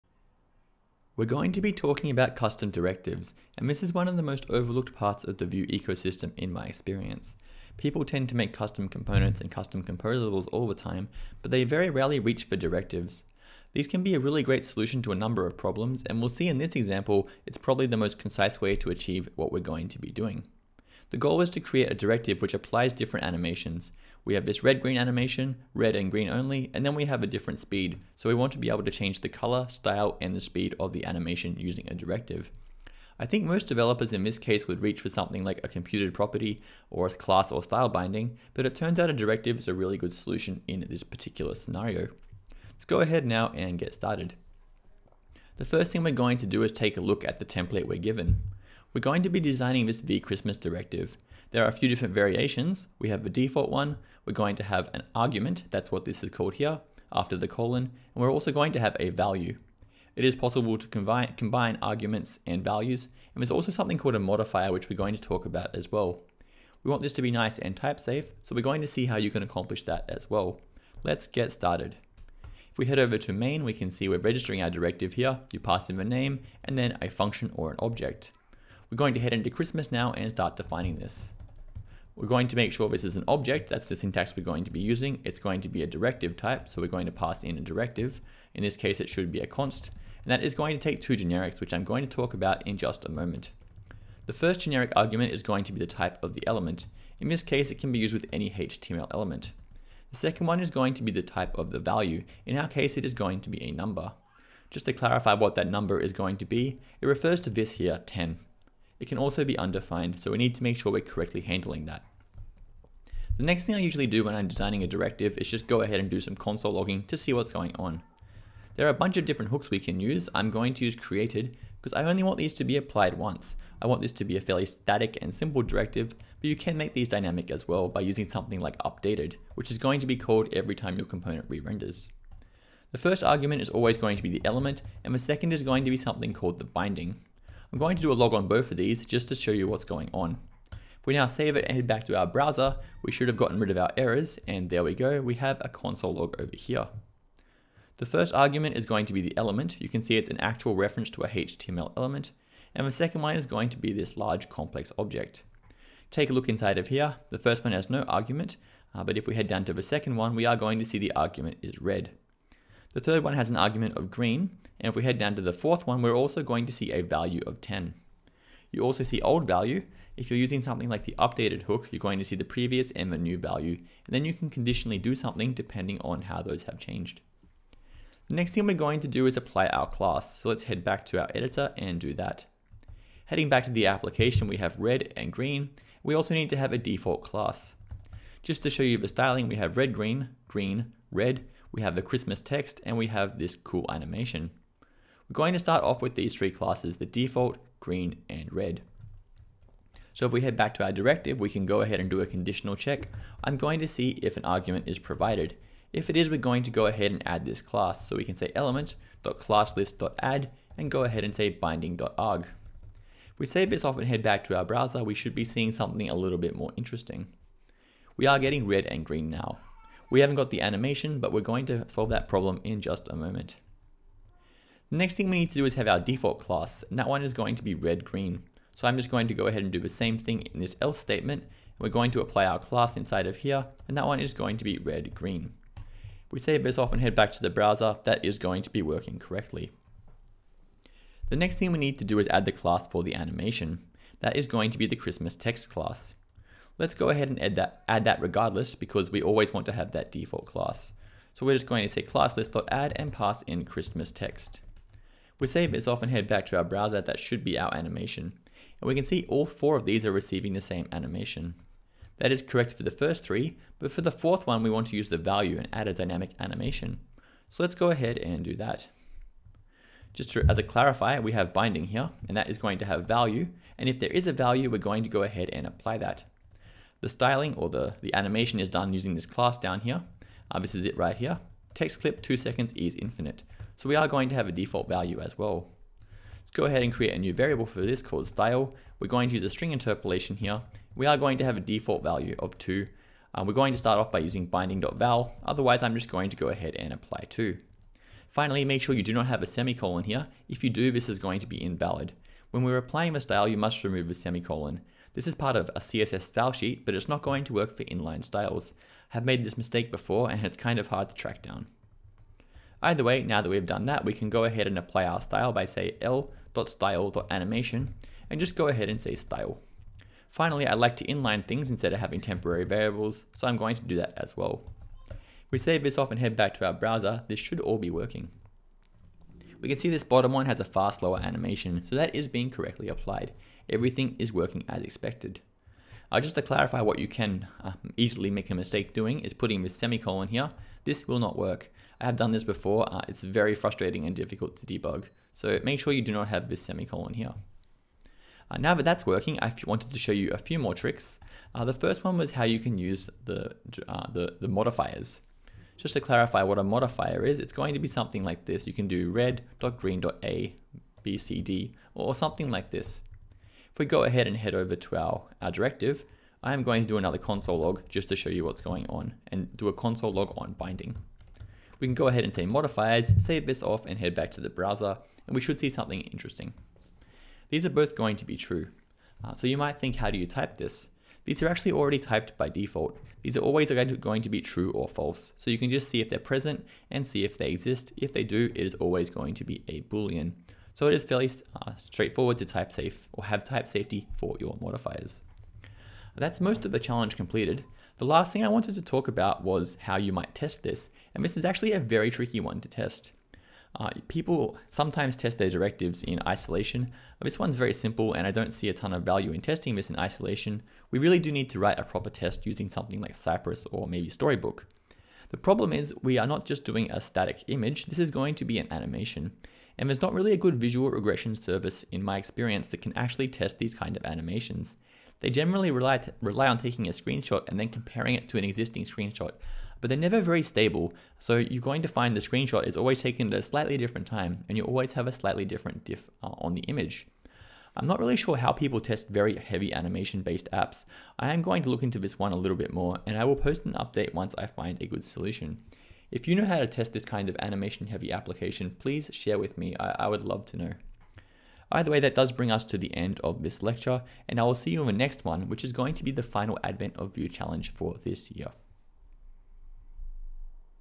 Type Safe Custom Directives (Advent of Vue #16)
My solution to the 16th Advent of Vue challenge! Pretty quick one, and something that doesn't get as much attention as it probably deserves - custom directives.

If you like my teaching style, I have a book and two courses!